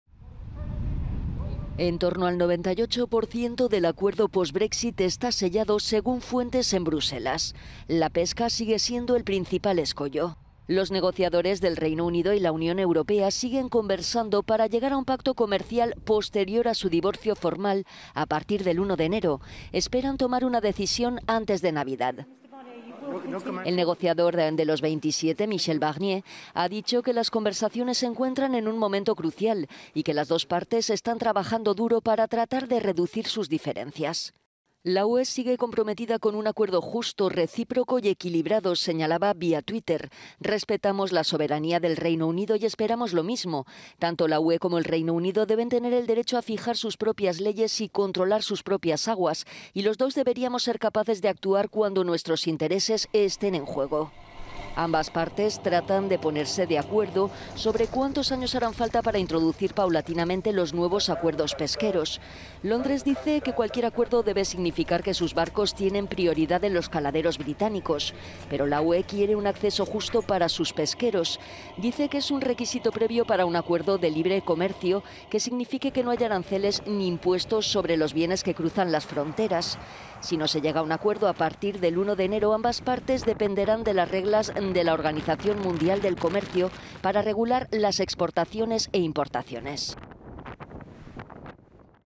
Brexit: a vueltas con la pesca
Es el principal escollo en las negociaciones finales para lograr un acuerdo sobre las relaciones futuras entre el Reino Unido y la Unión Europea, una vez consumado el Brexit, tras finalizar el período transitorio el 31 de diciembre.